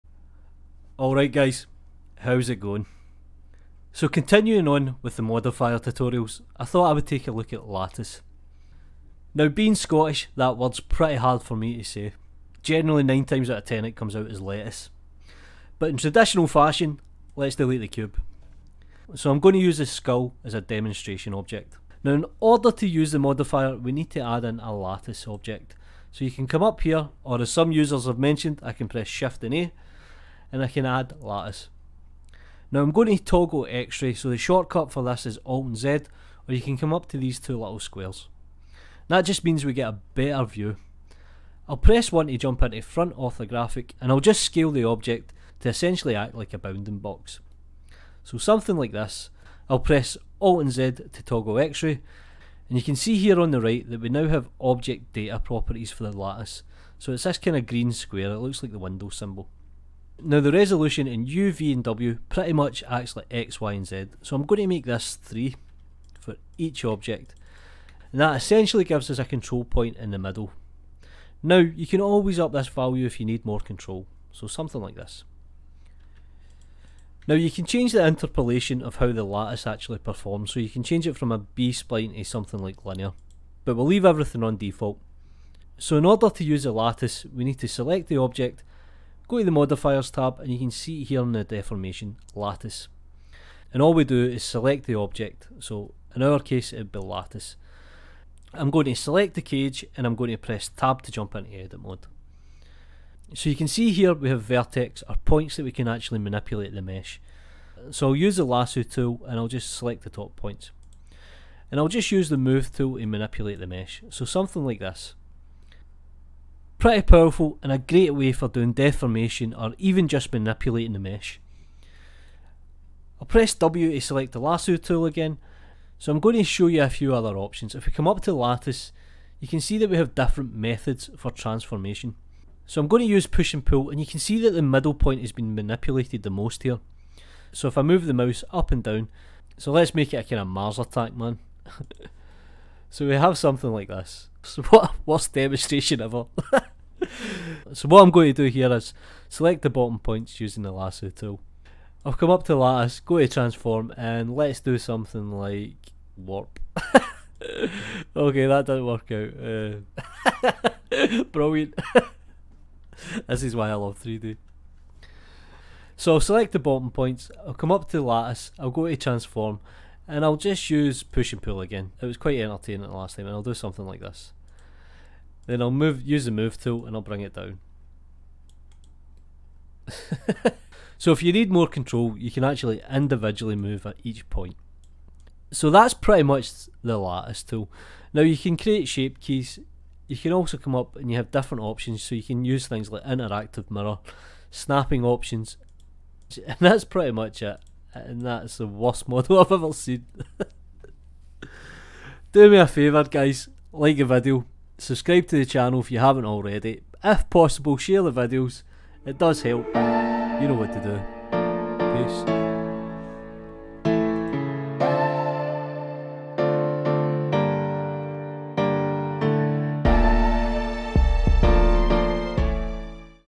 Blender 2.8 Tutorial: Lattice Modifier
Setup a lattice cage and modifier to manipulate the mesh

Lattice Modifier
The Lattice modifier deforms the base object according to the shape of a Lattice object. Objects to be deformed can be meshes, curves, surfaces, text, lattices and even particles.